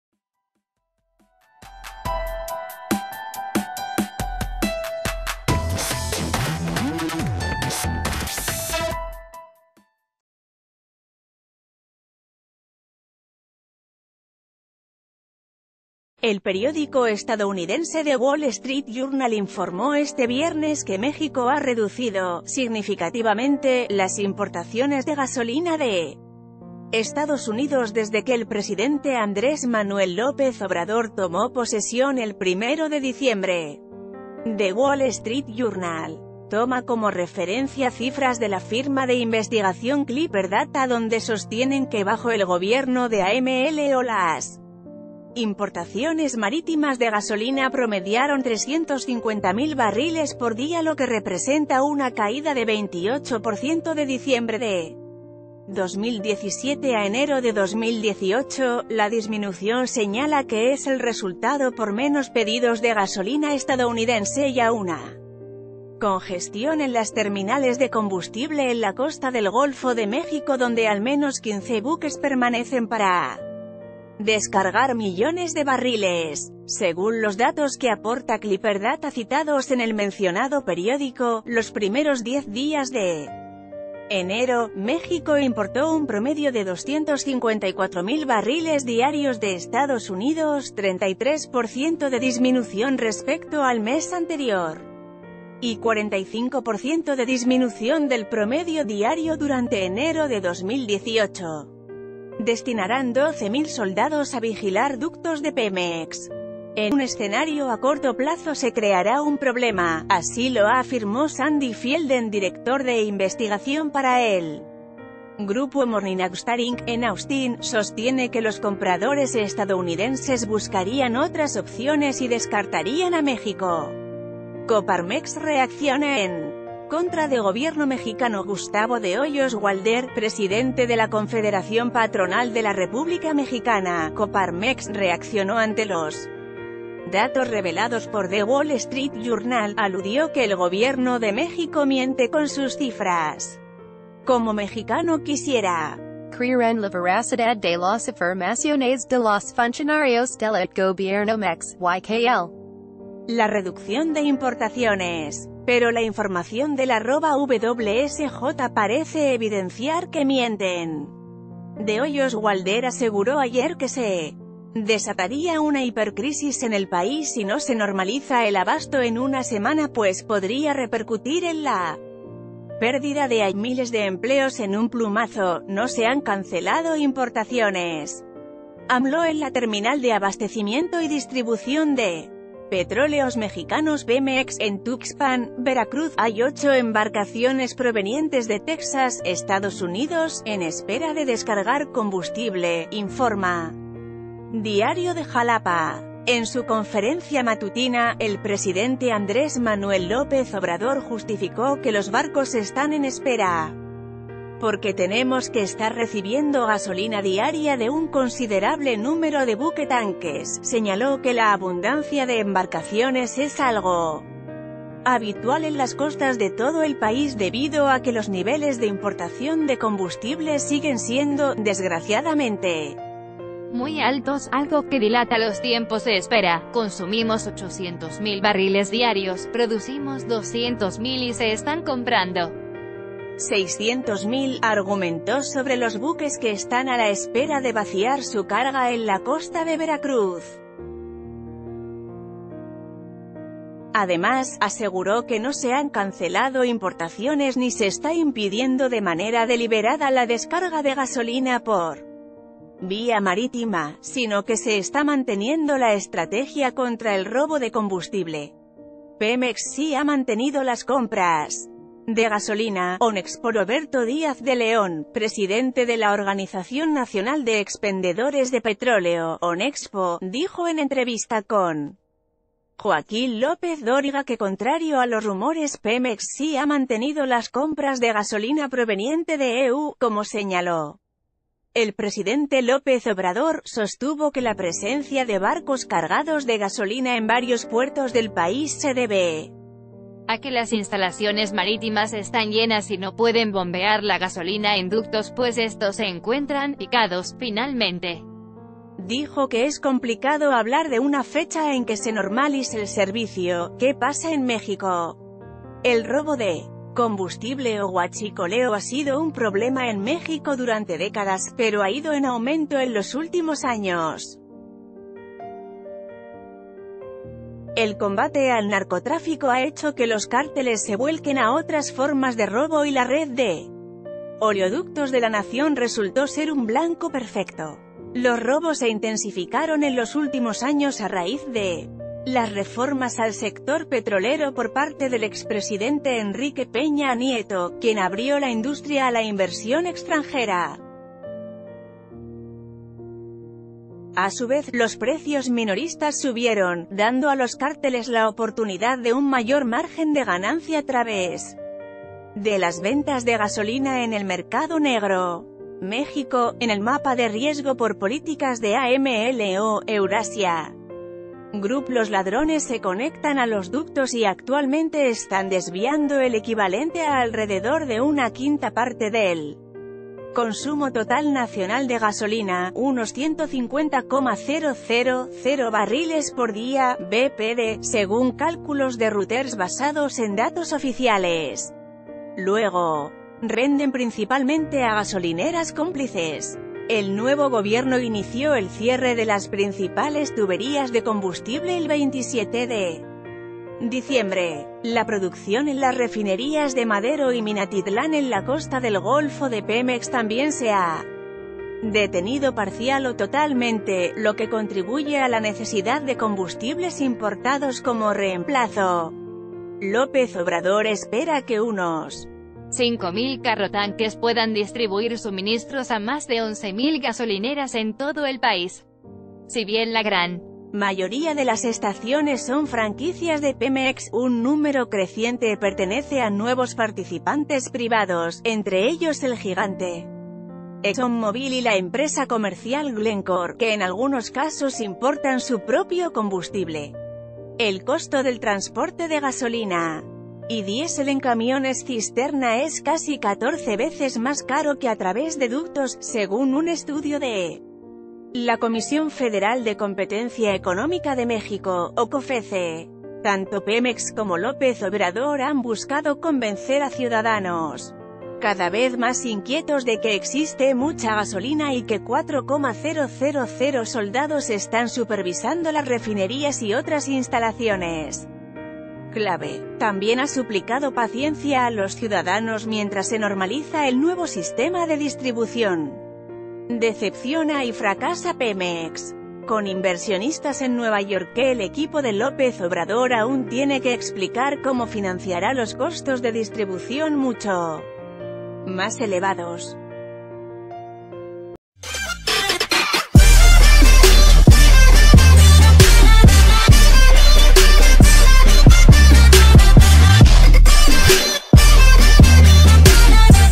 México redujo importaciones de gasolina de EU: WSJ
El periódico estadounidense The Wall Street Journal informó este viernes que México ha reducido "significativamente" las importaciones de gasolina de Estados Unidos desde que el presidente Andrés Manuel López Obrador tomó posesión el 1 de diciembre. The Wall Street Journal toma como referencia cifra...